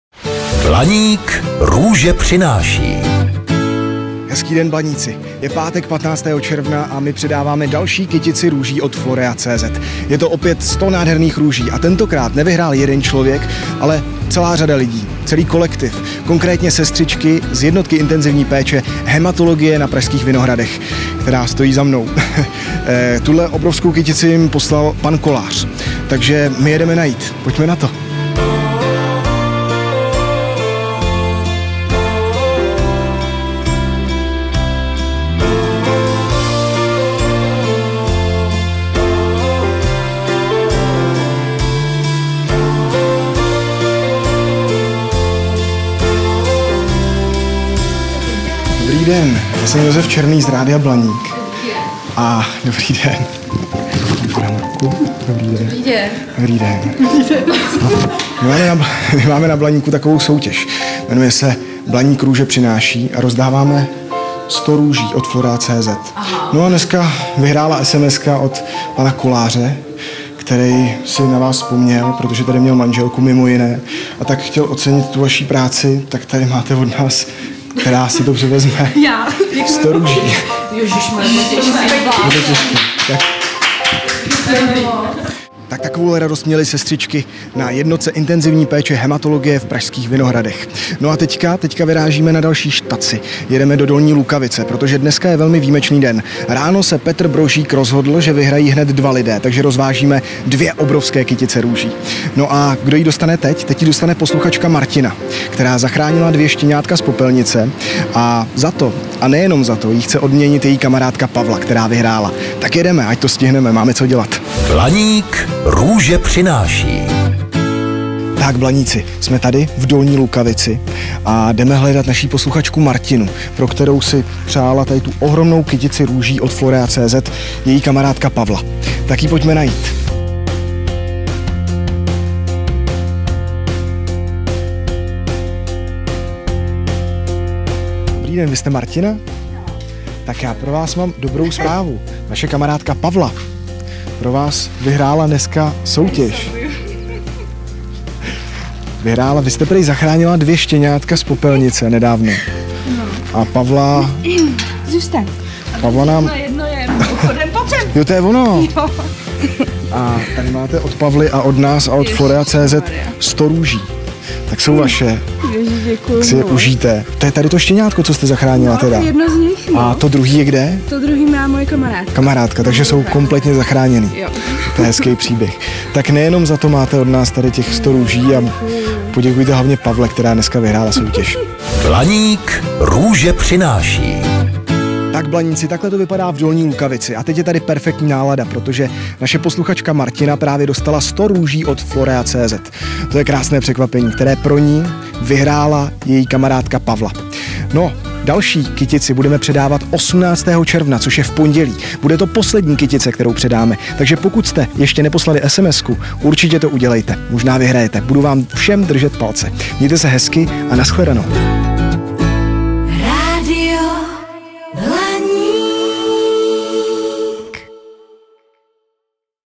BLANÍK růže přináší (15.6.2018)
Blaníci, tentokrát jsme zvládli potěšit obřími pugety od Florea.cz hned dva výherce. A komu jsme 2x 100 růží odvezli? Podívejte se na video, byla to paráda. :-)